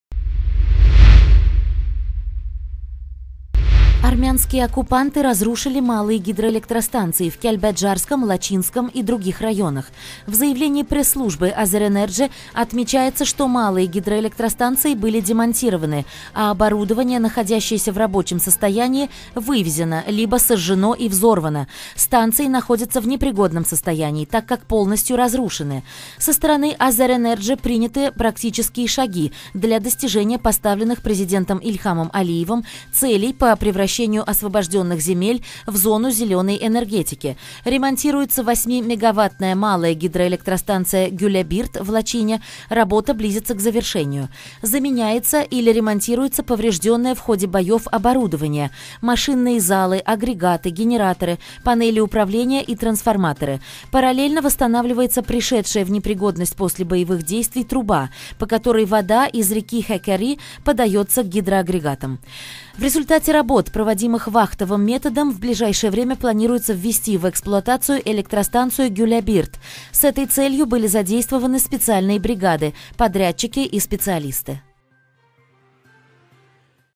В Лачине реконструируется ГЭС
Армянские оккупанты разрушили малые гидроэлектростанции в Кяльбаджарском, Лачинском и других районах.
В заявлении пресс-службы «Azərenerji» отмечается, что малые ГЭС были демонтированы, а оборудование, находящееся в рабочем состоянии вывезено, либо сожжено и взорвано - станции находятся в непригодном состоянии, так как полностью разрушены.
Со стороны «Azərenerji» предприняты практические шаги для достижения поставленных Президентом Ильхамом Алиевым целей по превращению освобожденных земель в зону зеленой энергетики - ремонтируется 8-мегаватная малая гидроэлектростанция «Гюлебирд» в Лачине, работа близится к завершению.
Заменяется или ремонтируется поврежденное в ходе боев оборудование - машинные залы, агрегаты, генераторы, панели управления и трансформаторы; параллельно восстанавливается пришедшая в непригодность после боевых действий труба, по которой вода из реки Хекери подается к гидроагрегатам.
В результате работ, проводимых вахтовым методом, в ближайшее время планируется ввести в эксплуатацию электростанцию «Гюлебирд» - с этой целью были задействованы специальные бригады, подрядчики и специалисты.